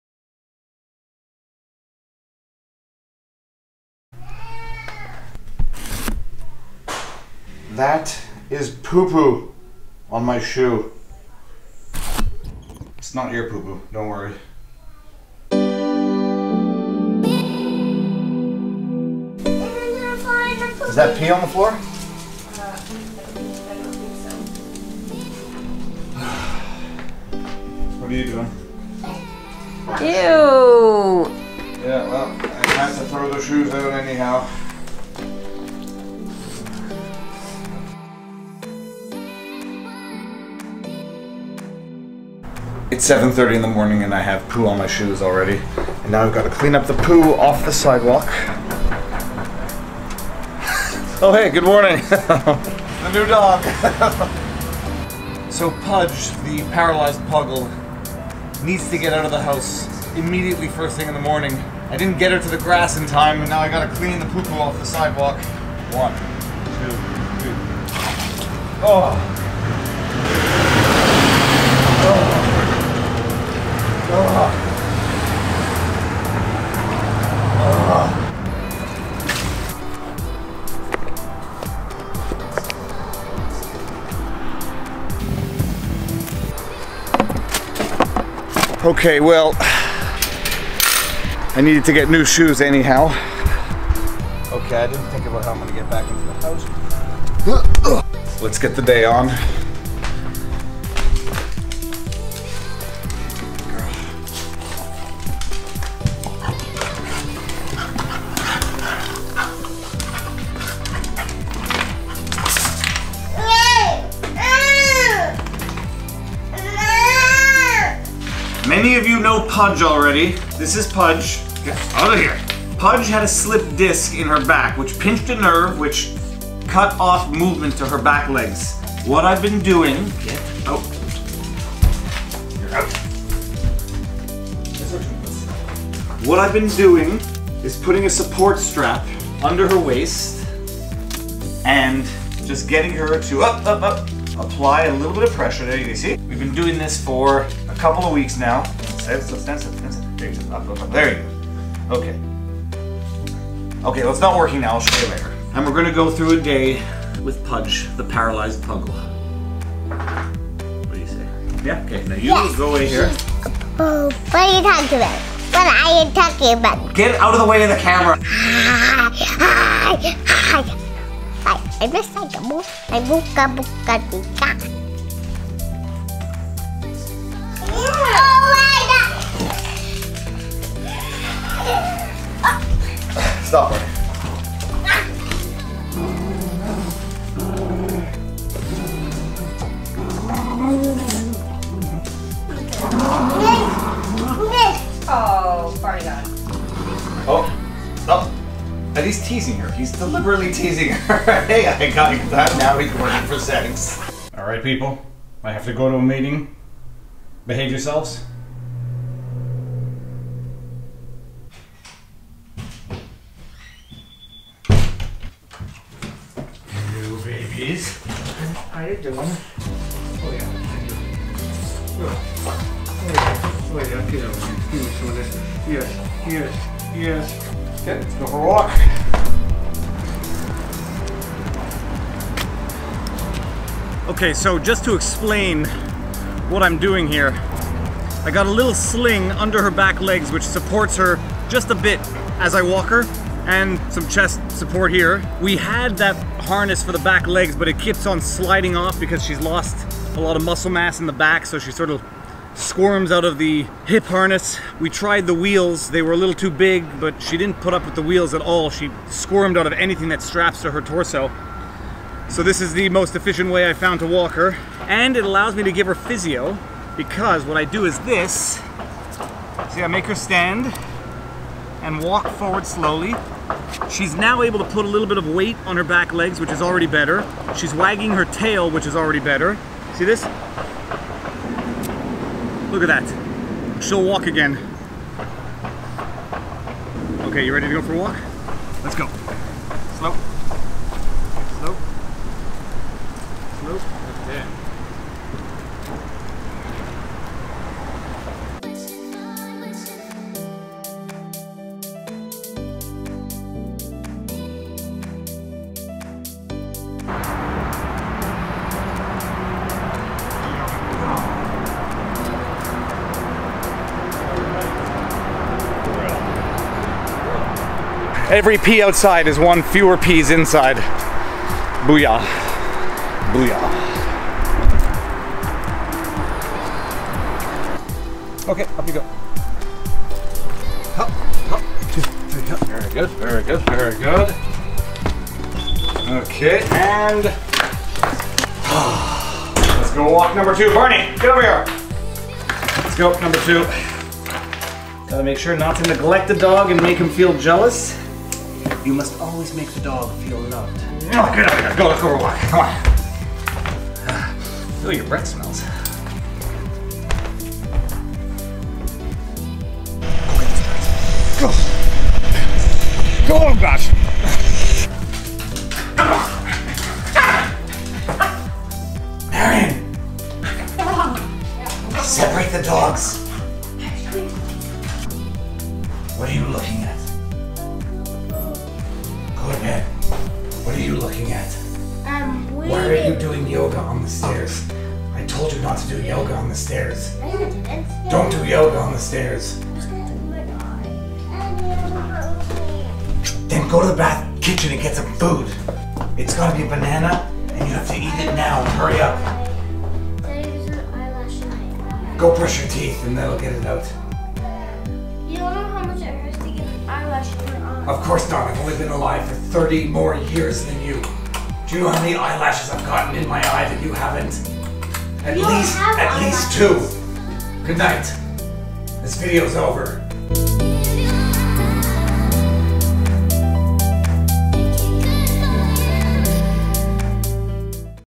LIFE OF PUDGE (THE PARALYZED PUGGLE)
6 months ago, Pudge got a slipped disc in her back. No traumatic accident - just a random wrong movement that caused a disc to slip pinch a nerve, and paralyze her back legs. We've been trying some physiotherapy, been giving her some extra attention, and we've been seeing some improvement (albeit very slow). This is a day in the life. And peace out! :)